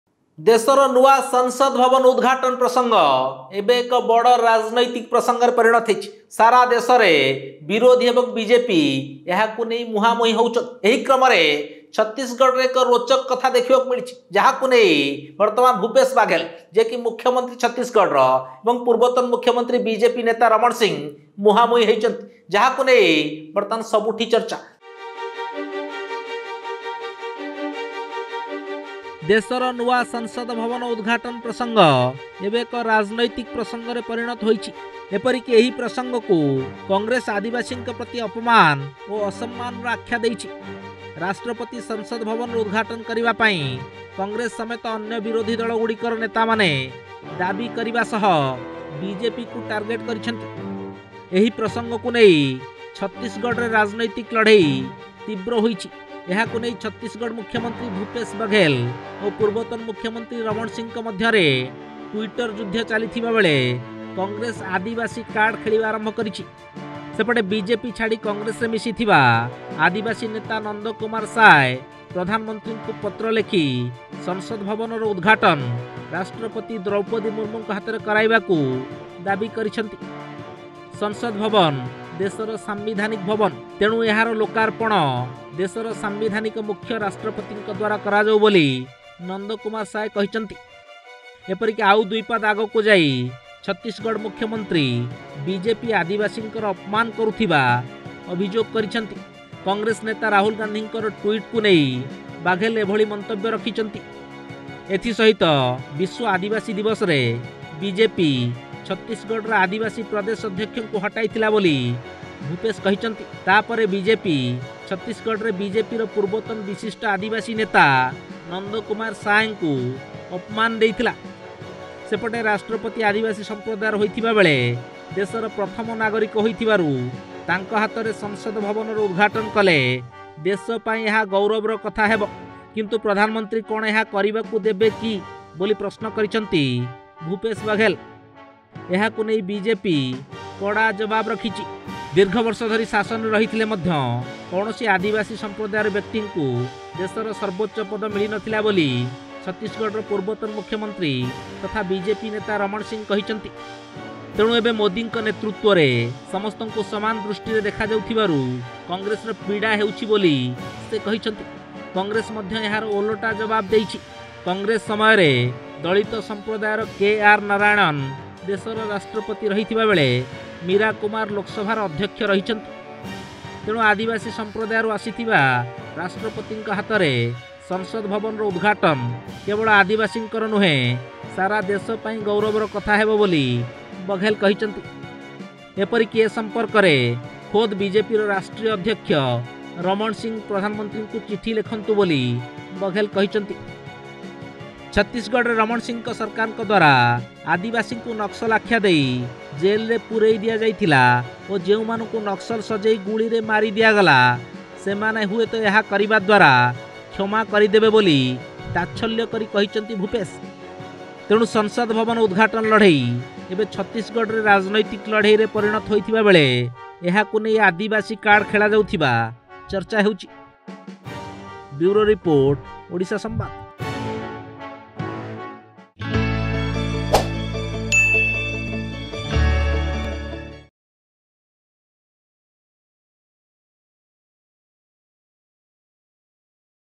The Politics of Chhattisgarh Has Intensified over the Inauguration Issue of the New Parliament
ଦେଶର ନୂଆ ସଂସଦ ଭବନ ଉଦଘାଟନ ପ୍ରସଙ୍ଗ ଏବେ ଏକ ରାଜନୈତିକ ପ୍ରସଙ୍ଗରେ ପରିଣତ ହୋଇଛି। ଏପରିକି ଏହି ପ୍ରସଙ୍ଗକୁ କଂଗ୍ରେସ ଆଦିବାସୀଙ୍କ ପ୍ରତି ଅପମାନ ଓ ଅସମ୍ମାନର ଆଖ୍ୟା ଦେଇଛି। ରାଷ୍ଟ୍ରପତି ସଂସଦ ଭବନର ଉଦଘାଟନ କରିବା ପାଇଁ କଂଗ୍ରେସ ସମେତ ଅନ୍ୟ ବିରୋଧୀ ଦଲଗୁଡିକର ନେତାମାନେ ଦାବି କରିବା ସହ ବିଜେପିକୁ ଟାର୍ଗେଟ କରିଛନ୍ତି